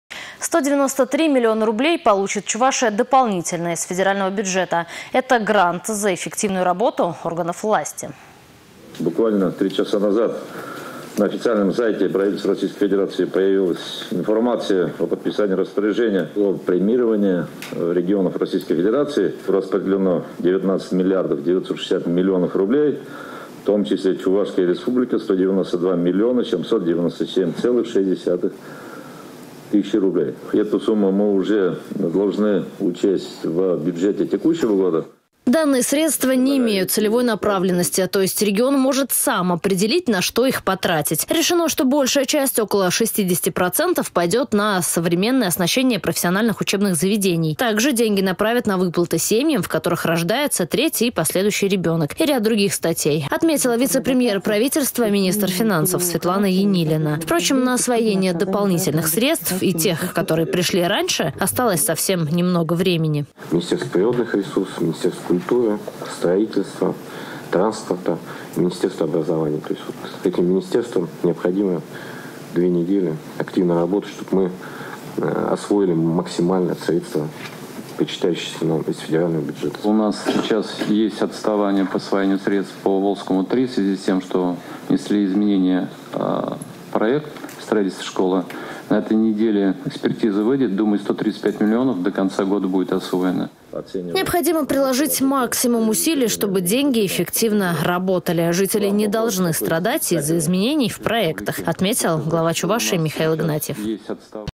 193 миллиона рублей получит Чувашия дополнительно из федерального бюджета.
Это грант за эффективную работу органов власти.